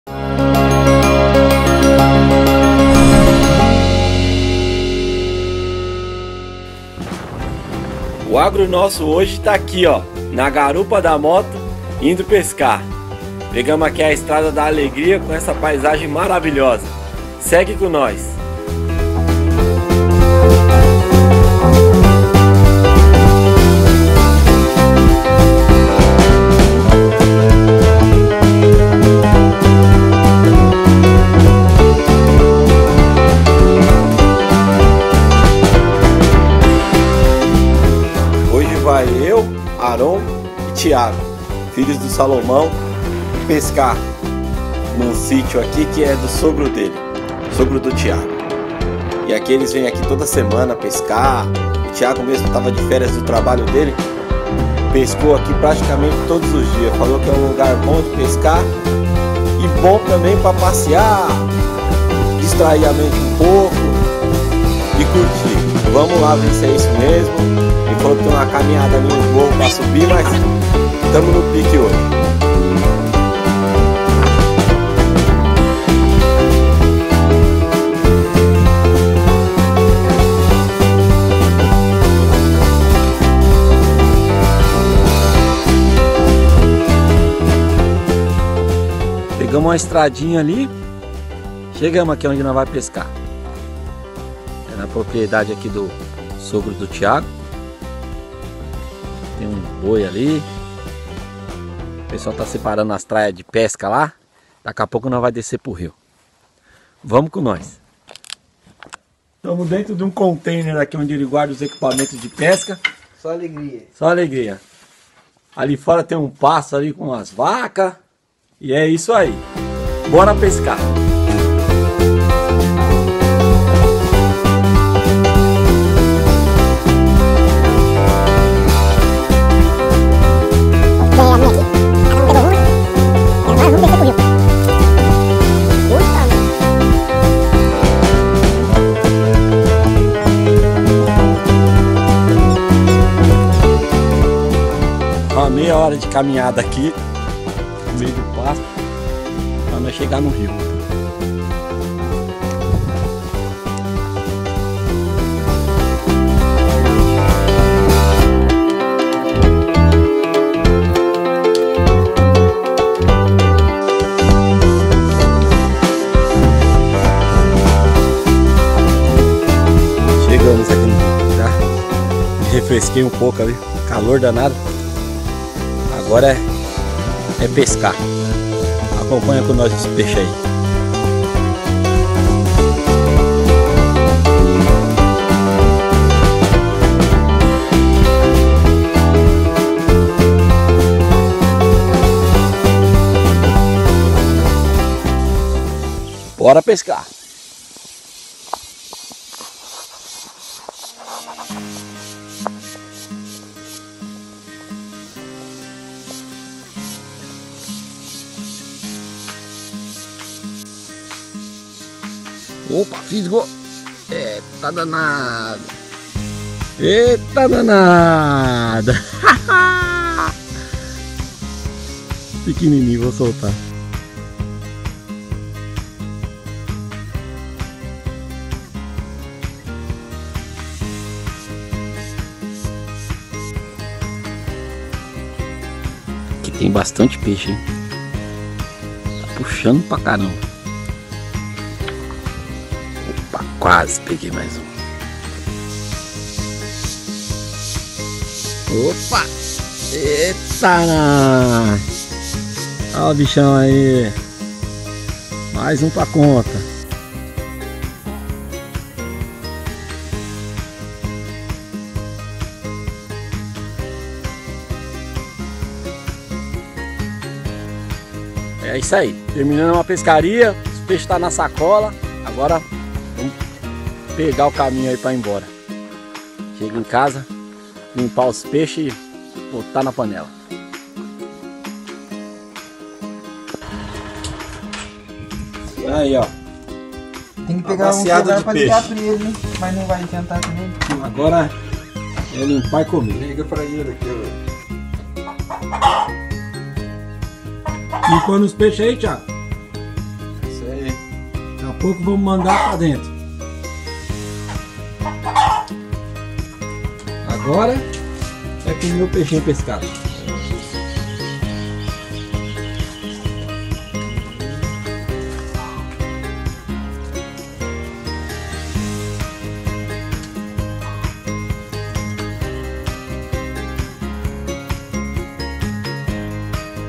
Dia de pesca em Mandaguari PR | Rio Araçu
O Agro Nosso tirou o dia de folga para pescar no Rio Araçu.
Se liga na aventura!